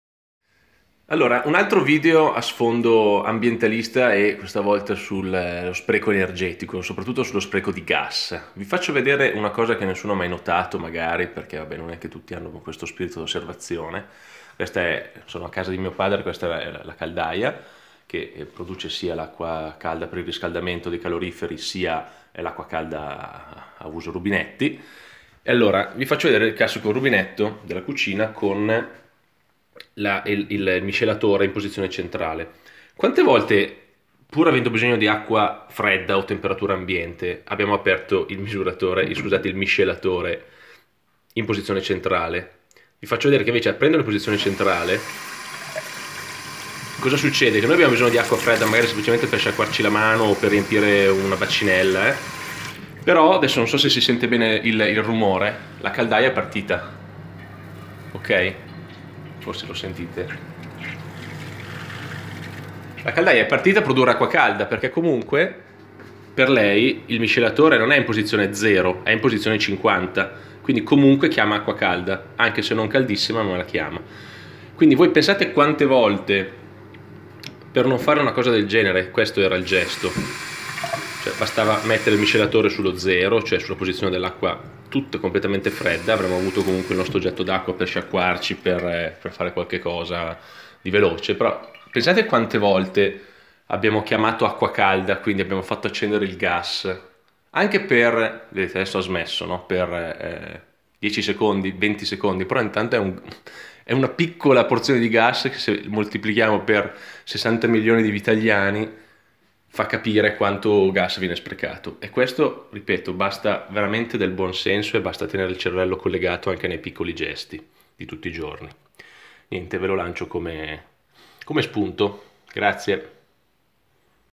Un piccolo gesto per non sprecare gas inutilmente
Ci avete mai fatto caso? Basta un po' di attenzione in più e un piccolo movimento della mano per risparmiare gas in casa.